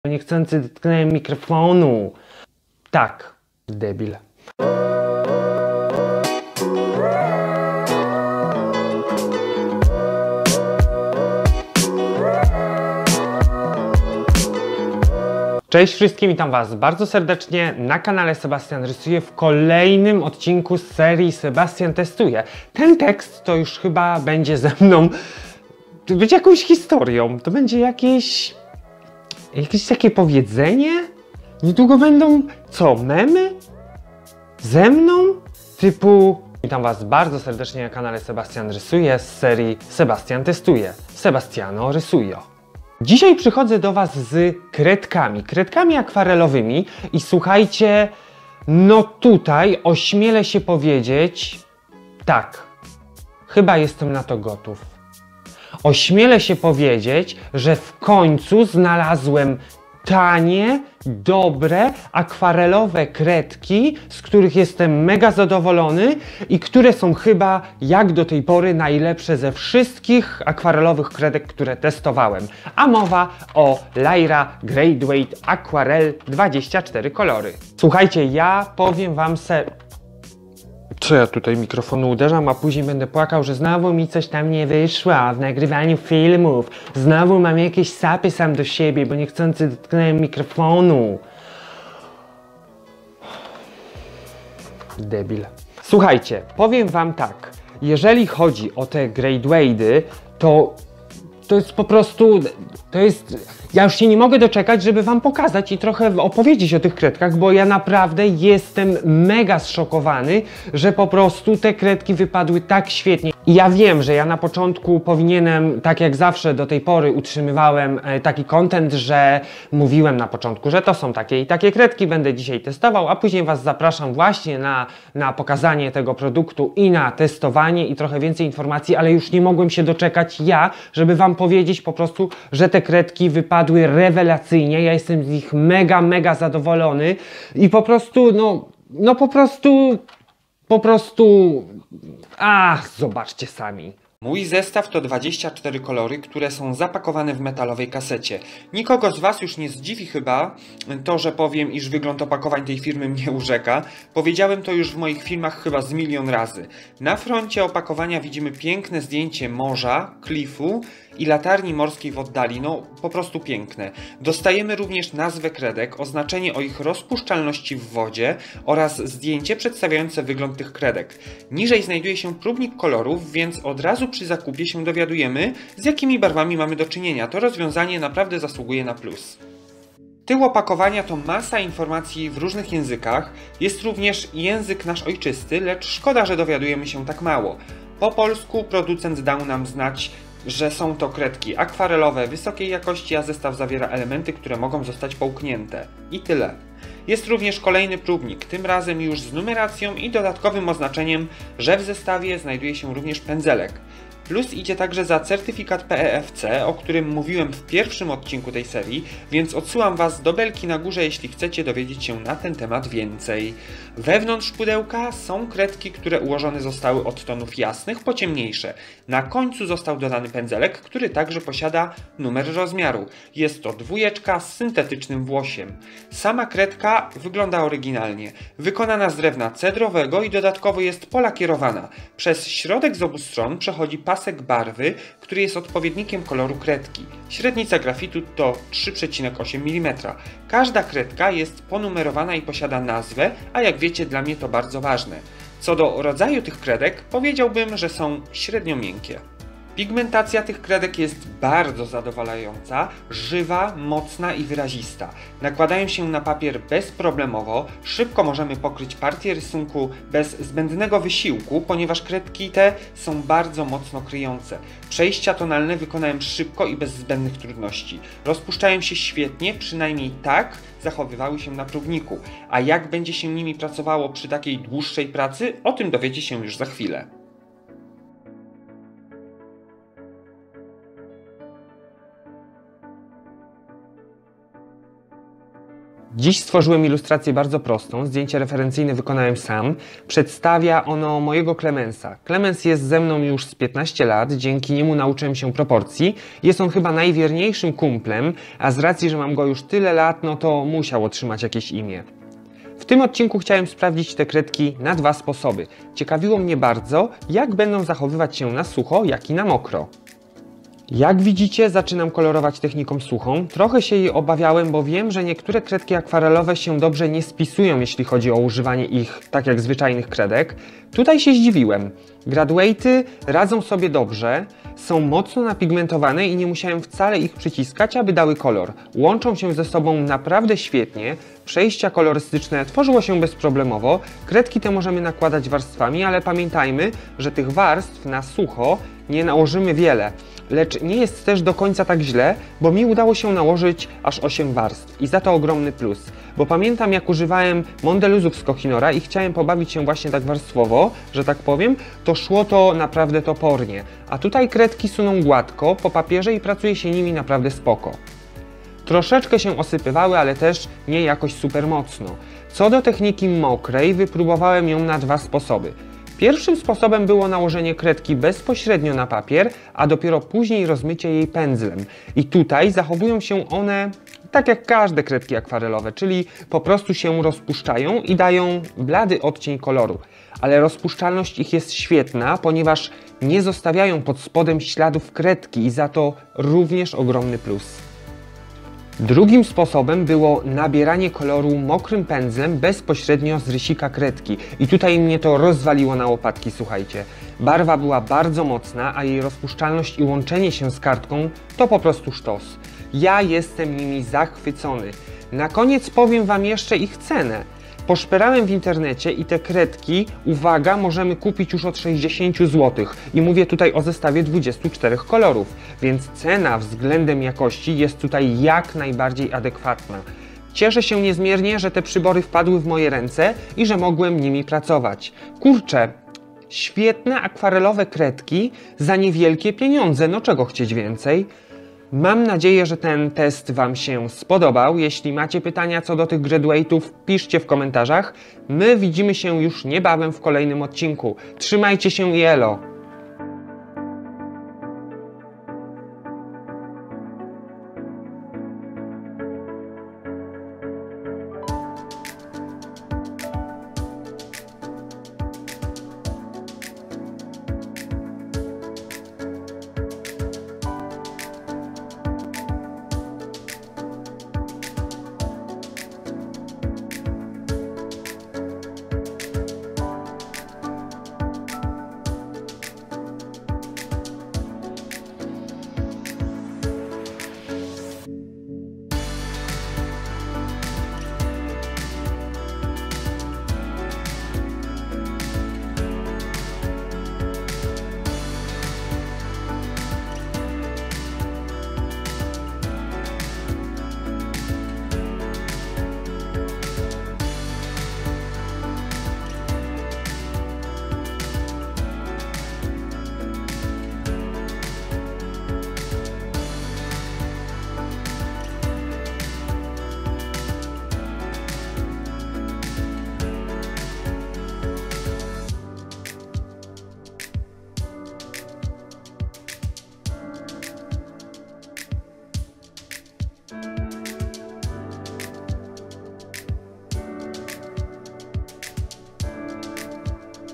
Lyra Graduate AQUARELL 🤯 Najlepsze TANIE kredki akwarelowe?!
Tak! Dobrze czytacie tytuł!
W dzisiejszym odcinku pokaże Wam kredki które są rozpuszczalne w wodzie,a dodatkowo nie kosztują masę pieniędzy!
Jestem po prostu zaskoczony efektami jakie dają Graduate Od Lyry!
Koniecznie zobaczcie!

Odcinek troche "podrasowany". Staram się dodawać co jakis czas coś nowego,aby się Wam milej oglądało.
Nie jestem MISZCZEM w edycji filmów ale doceńcie moje starania :)

Zapoznajcie sie też z innymi produktami Lyra!
Oglądając moje filmy bądź przeglądnijcie katalogi ze strony Producenta! :)